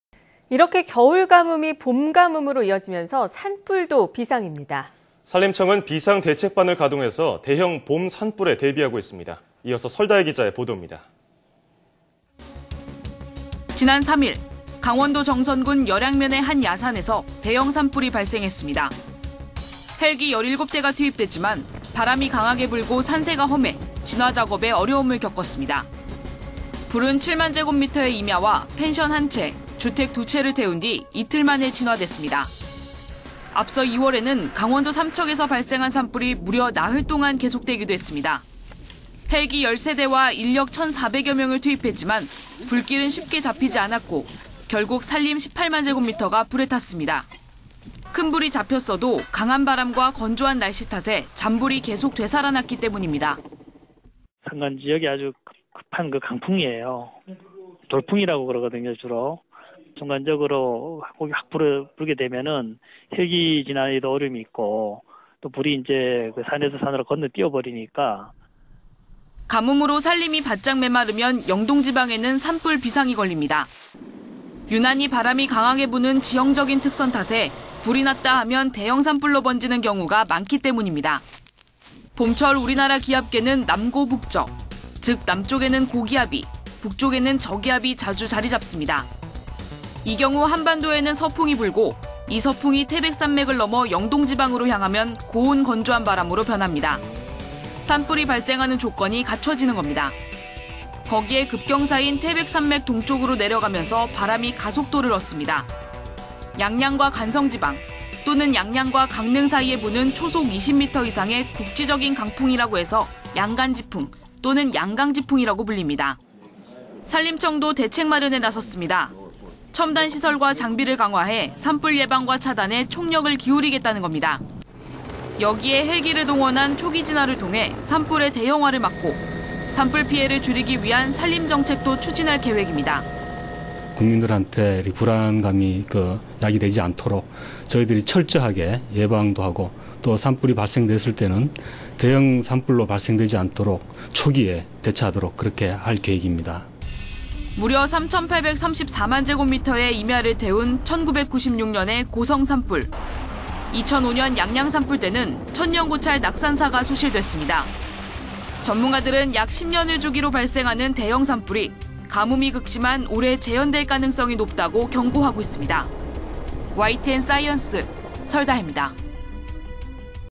메마른 산야…대형산불 비상 / YTN 사이언스
[앵커]
겨울 가뭄이 봄 가뭄으로 이어지면서 산불도 비상입니다.

산림청은 비상 대책반을 가동해 대형 봄 산불에 대비하고 있습니다.

설다혜 기자가 보도합니다.

[기자]
지난 3일.

강원도 정선군 여량면의 한 야산에서 대형 산불이 발생했습니다.

헬기 17대가 투입됐지만 바람이 강하게 불고, 산세가 험해 진화작업에 어려움을 겪었습니다.

불은 7만 제곱미터의 임야와 펜션 1채, 주택 2채를 태운 뒤 이틀 만에 진화됐습니다.

앞서 2월에는 강원도 삼척에서 발생한 산불이 무려 나흘동안 계속되기도 했습니다.

헬기 13대와 인력 1,400여 명을 투입했지만 불길은 쉽게 잡히지 않았고, 결국 산림 18만 제곱미터가 불에 탔습니다.

큰 불이 잡혔어도 강한 바람과 건조한 날씨 탓에 잔불이 계속 되살아났기 때문입니다.

[인터뷰:김정황, 동부지방산림청 산림재해안전과 보호팀장]
"산간지역의 급한 강풍이에요 돌풍이라고 그러거든요. 순간적으로 불이 확 불면 헬기진화에도 어려움이 있고 불이 또 산에서 산으로 건너뛰어버리니까..."

가뭄으로 산림이 바짝 메마르면 영동지방에는 산불 비상이 걸립니다.

유난히 바람이 강하게 부는 지형적인 특성 탓에 불이 났다하면 대형산불로 번지는 경우가 많기 때문입니다.

봄철 우리나라 기압계는 남고북저, 즉 남쪽에는 고기압이 북쪽에는 저기압이 자주 자리잡습니다.

이 경우 한반도에는 서풍이 불고 이 서풍이 태백산맥을 넘어 영동지방으로 향하면 고온건조한 바람으로 변합니다.

산불이 발생하는 조건이 갖춰지는 겁니다.

거기에 급경사인 태백산맥 동쪽으로 내려가면서 바람이 가속도를 얻습니다.

양양과 간성지방, 또는 양양과 강릉 사이에는 부는 초속 20미터 이상의 국지적인 강풍이라고 해서 양간지풍, 또는 양강지풍이라고 불립니다.

산림청도 대책 마련에 나섰습니다.

첨단 시설과 장비를 강화해 산불 예방과 차단에 총력을 기울이겠다는 겁니다.

여기에 헬기를 동원한 초기진화를 통해 산불의 대형화를 막고, 산불피해를 줄이기 위한 산림 정책도 추진할 계획입니다.

[인터뷰:김용하, 산림청 차장]
"국민한테 불안감이 야기되지 않도록 저희가 철저하게 예방도 하고 또 산불이 발생됐을 때는 대형 산불로 발생되지 않도록 초기에 대처하도록 그렇게 할 계획입니다."

무려 3834만 제곱미터의 임야를 불태운 1996년의 ...